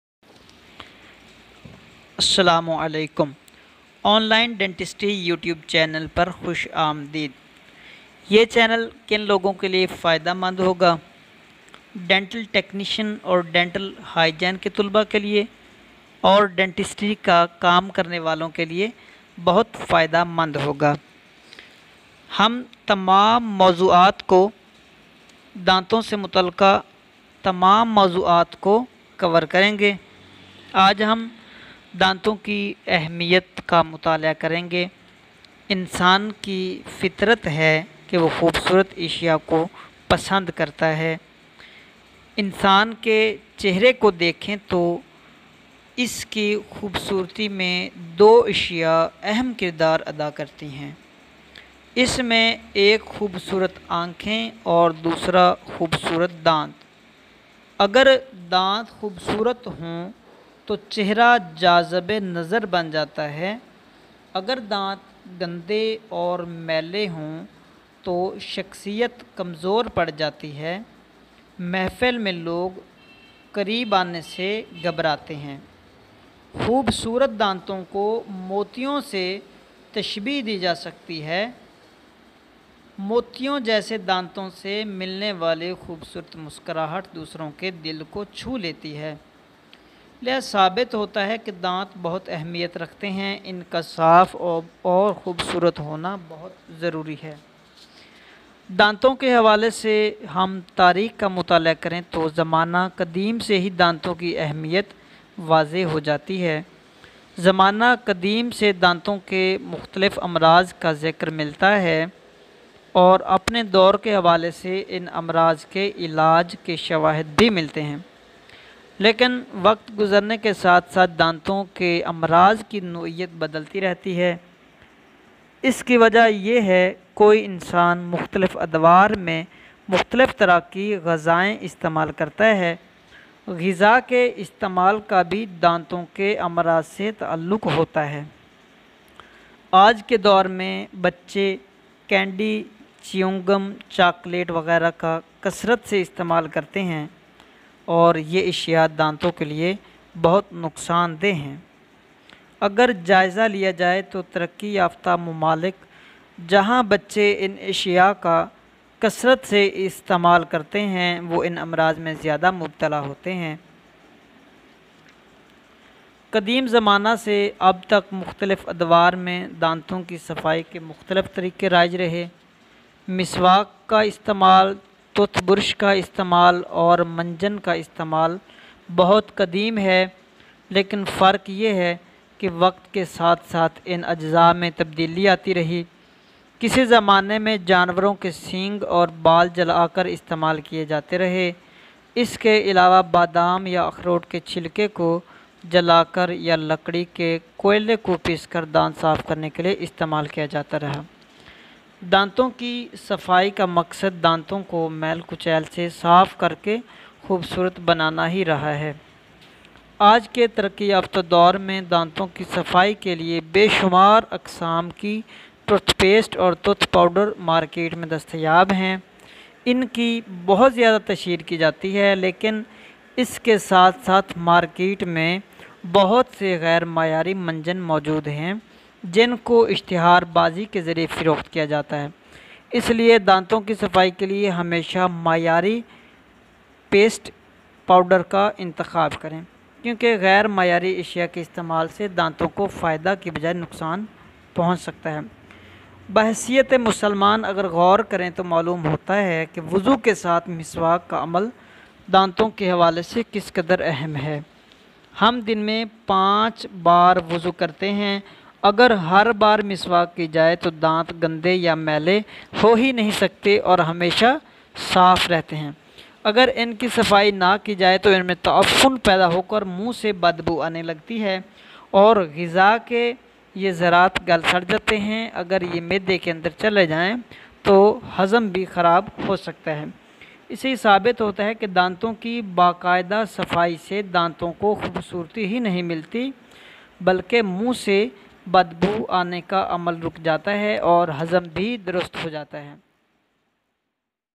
The importance of teeth/Danton ki Ahmeyat/Video.1/Modern Dentistry Online
ڈینٹل ٹیکنیشن اور ڈینٹسٹری کاکام کرنے والوں کےلئے بہترین وڈیوسیریز۔
Online Dental Institute

Dant hmari zindagi ma bohat ahmeyat rakhty hain.in ki hifazat krna hmari zimadari hai.